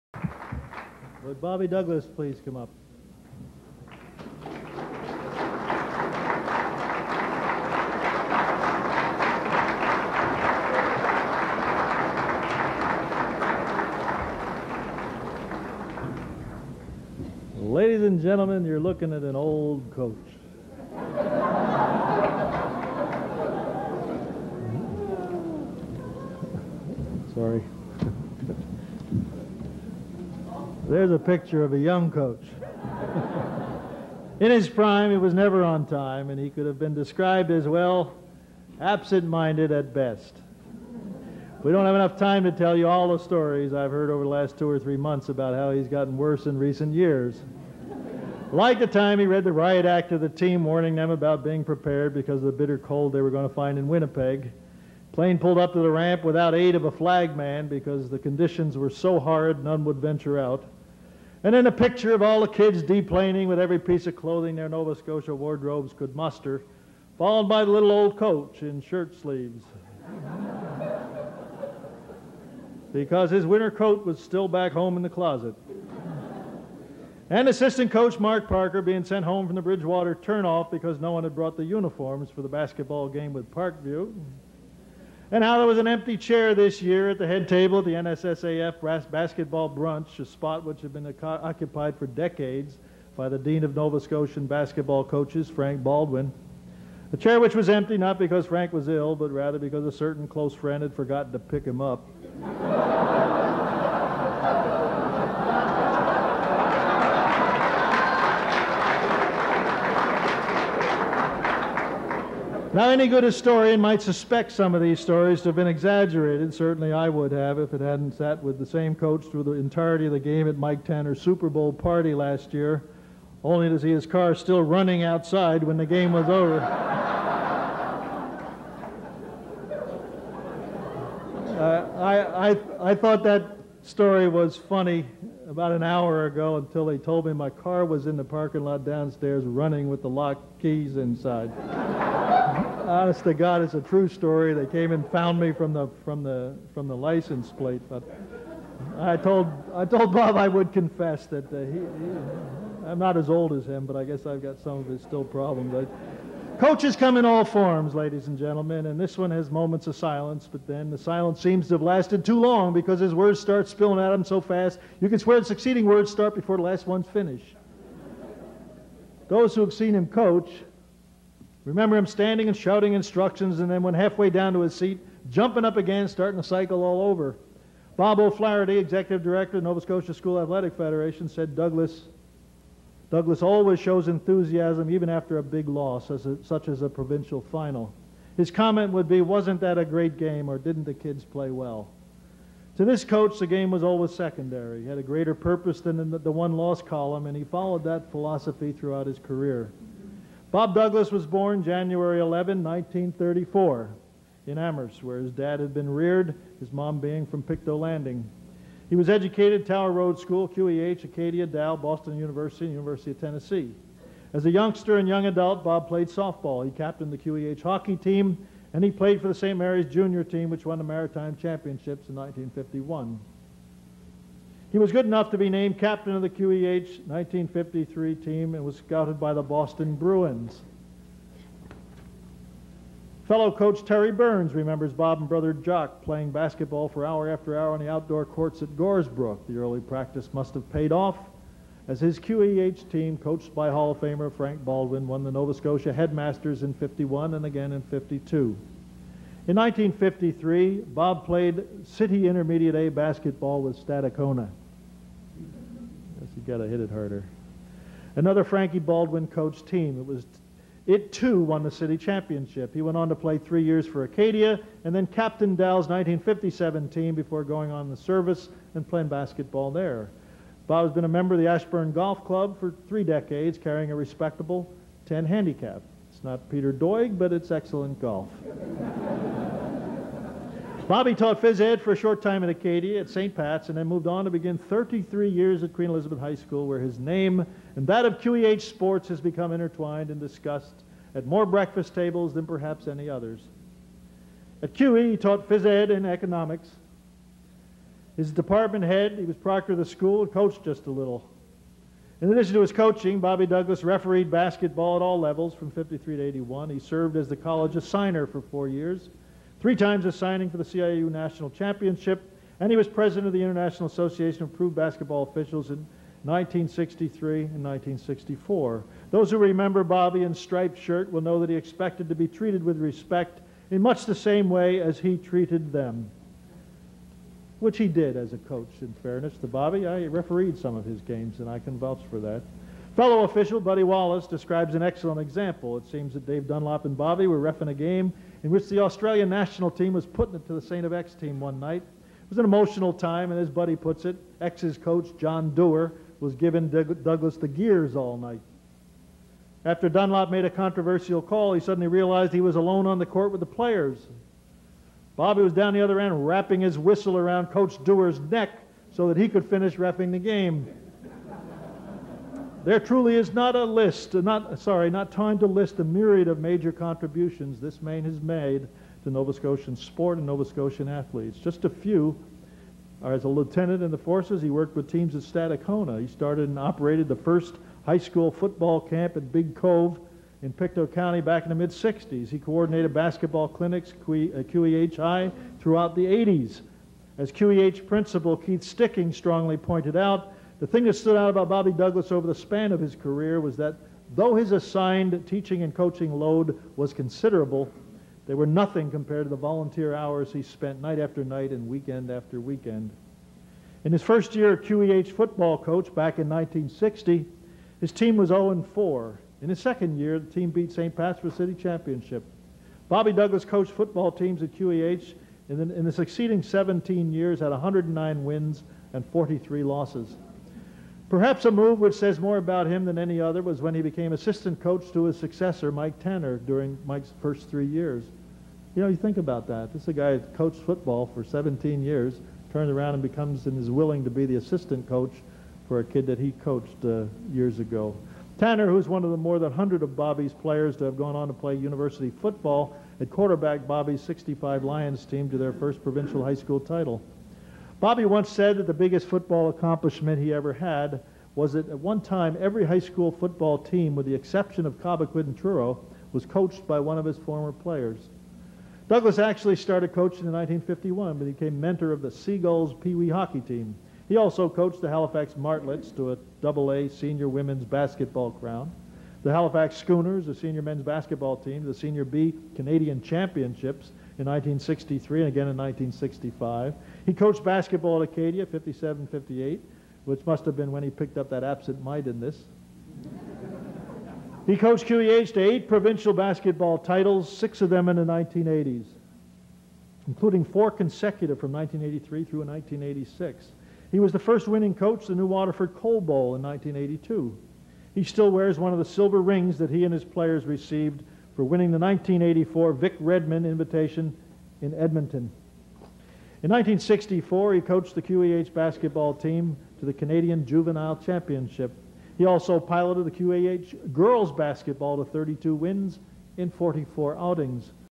Robert Bob Douglas Induction, 1994 Part 1
The Nova Scotia Sport Hall of Fame inducts Robert "Bob" Douglas